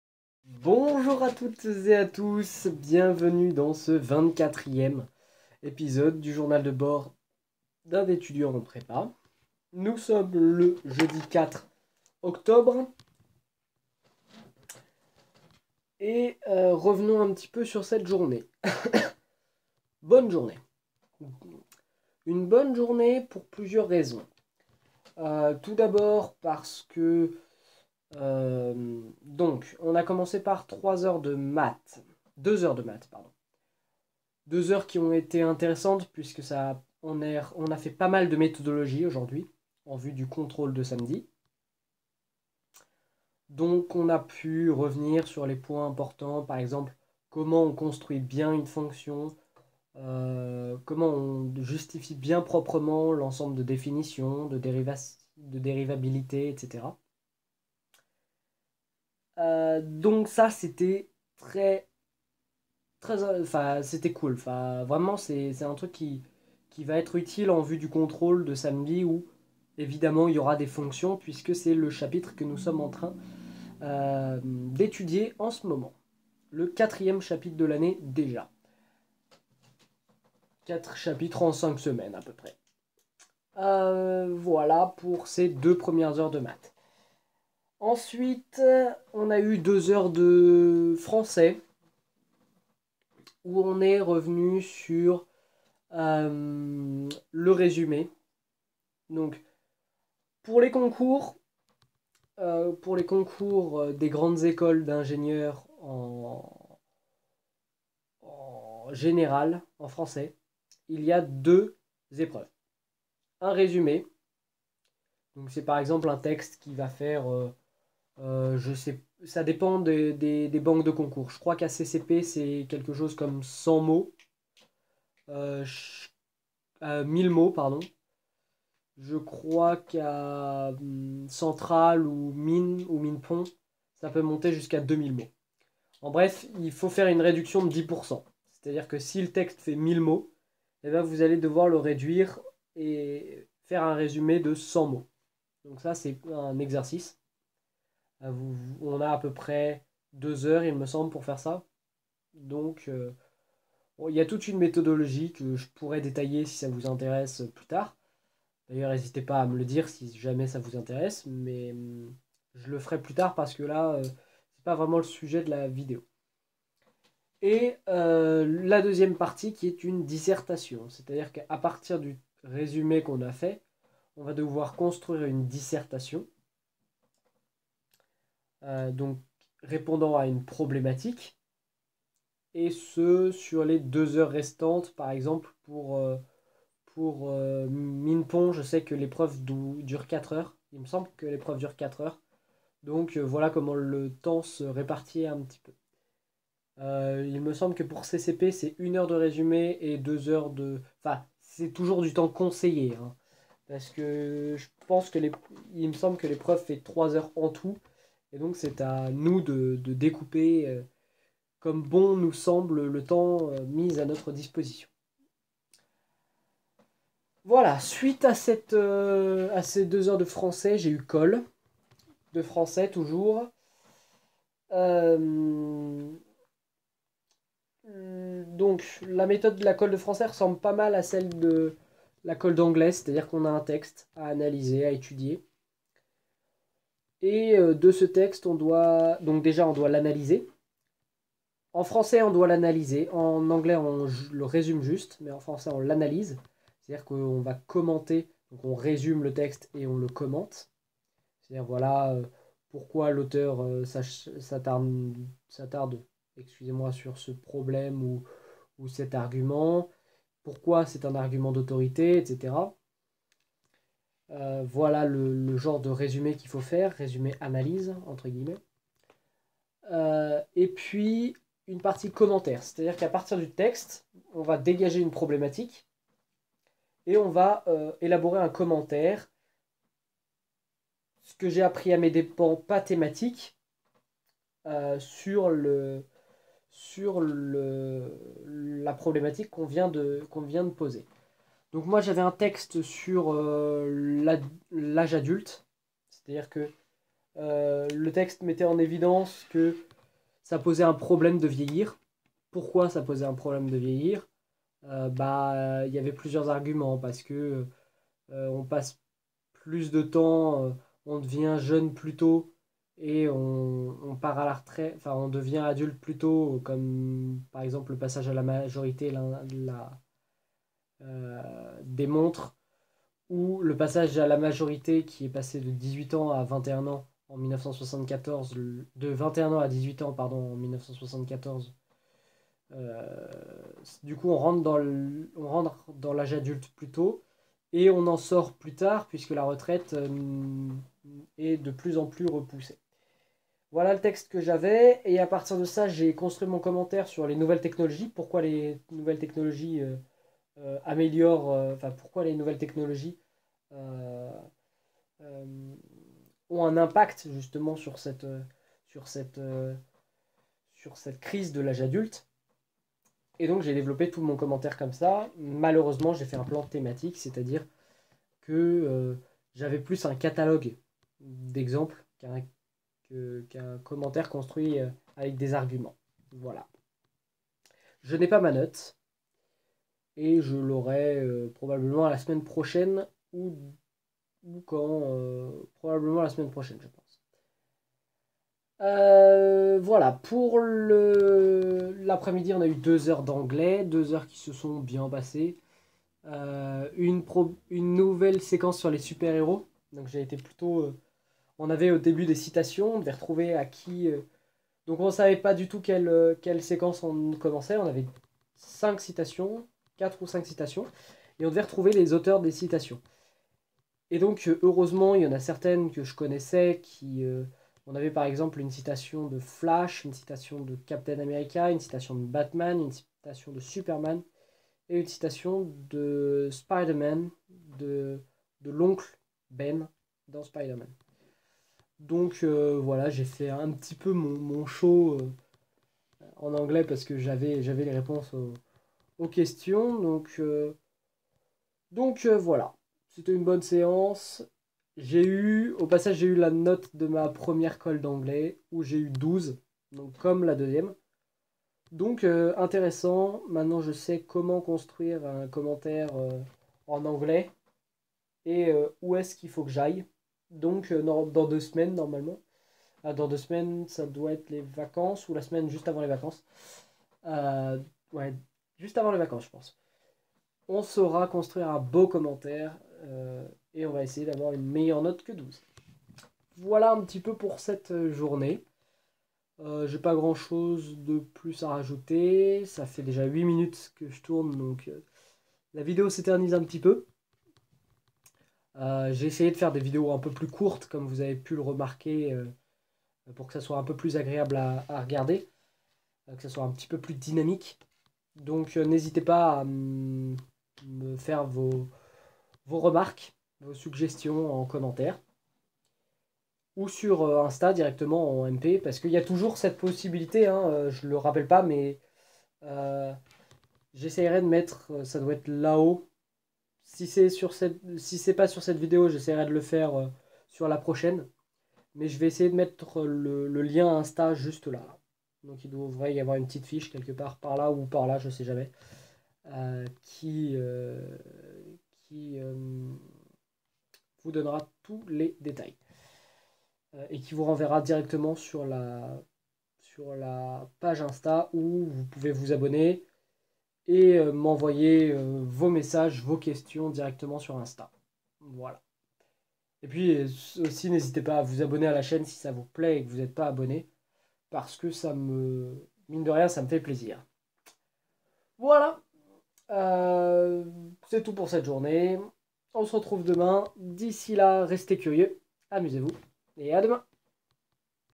Journal de Bord MPSI Jour 24
Une bonne journée, de la méthodologie et un 12 en colles d'anglais